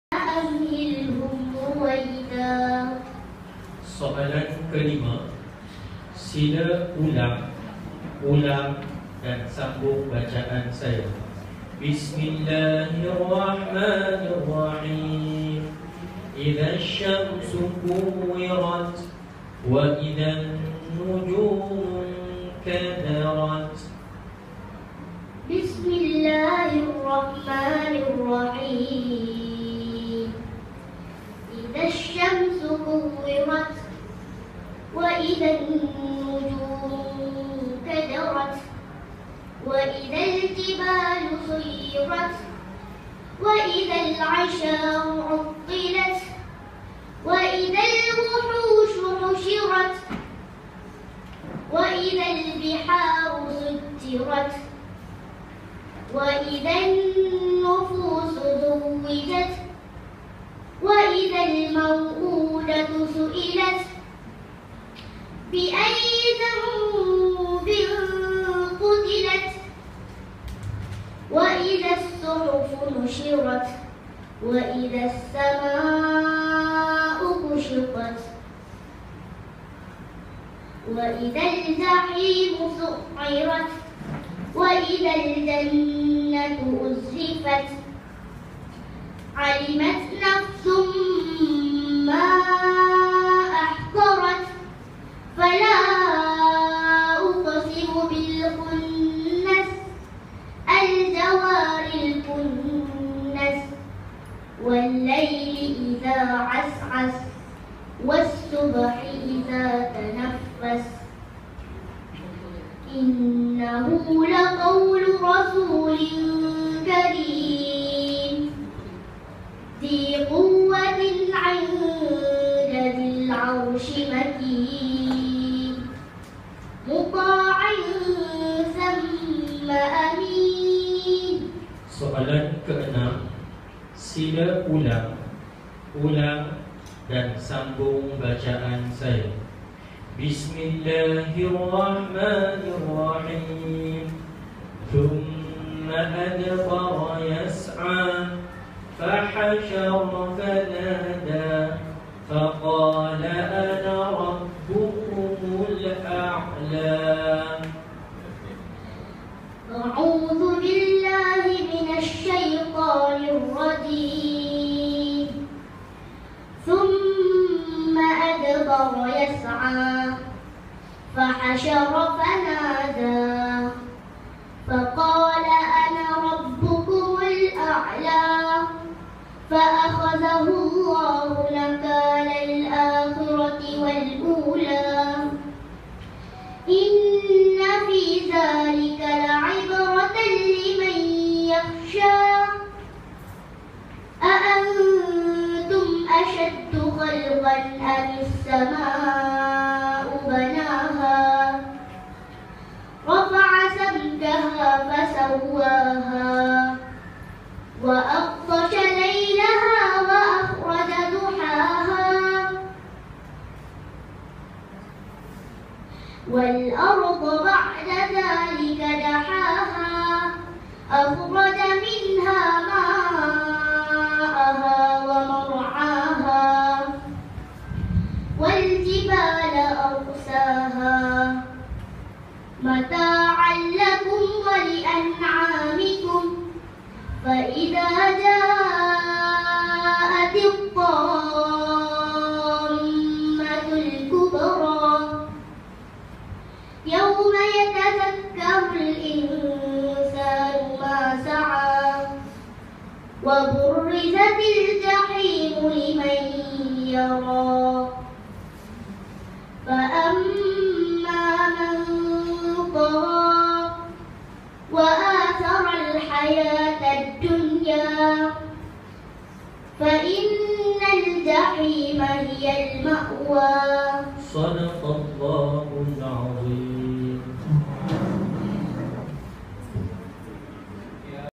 MTQ2017 Majlis Tadarus Al-Quran 2017 Peringkat Negeri Wilayah Persekutuan SK Pengkalan Tentera Darat
MTQ2017 Majlis Tadarus Al-Quran 2017 Peringkat Negeri Wilayah Persekutuan Kuala Lumpur, 8 April 2017 berlangsung di SK Pengkalan Tentera Darat, Kem Sungai Besi Kuala Lumpur

Pertandingan Bercerita Kisah Al-Quran,
Pertandingan Tilawah Al-Quran,
Pertandingan Hafazan.

SK Wangsa Maju Zon R10
SK Jalan Hang Tuah 2
SK Segambut Makmur
SK Taman Koperasi Polis
SK Tun Hussein Onn
SK Bukit Bandaraya
SK Taman Padang Tembak 2
SK Seri Tasik